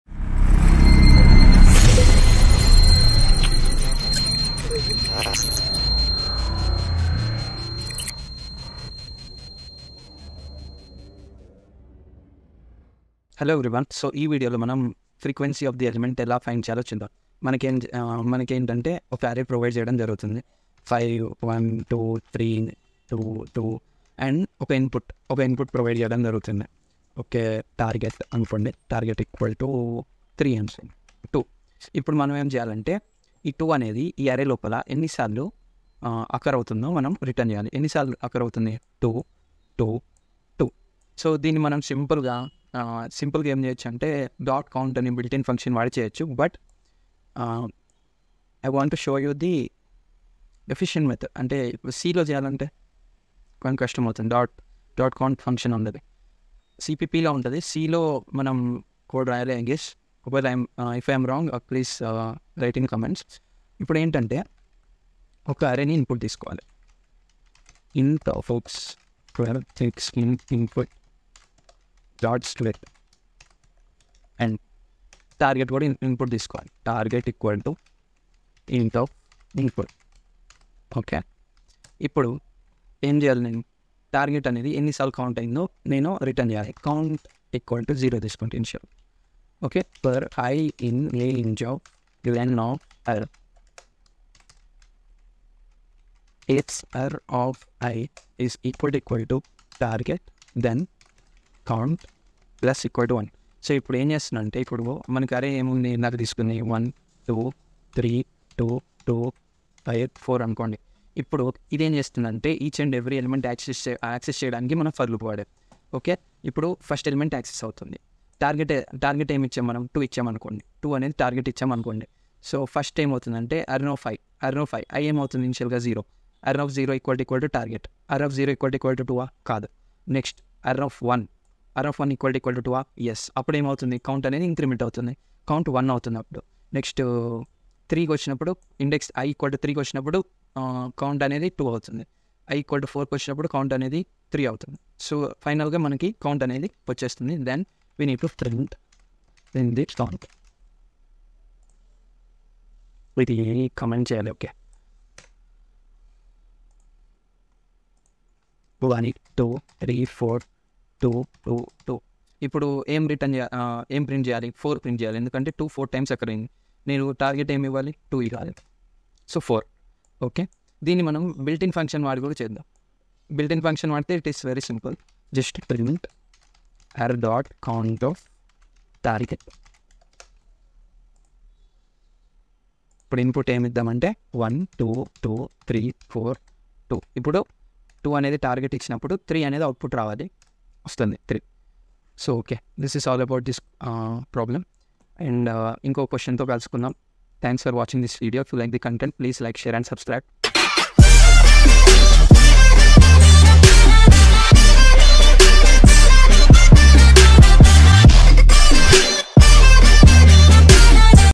Frequency of an element in an Array in PYTHON in TELUGU | ARRAYS | DataStructures by S.Javahar Reddy
solve this question in below links and increase your coding profiles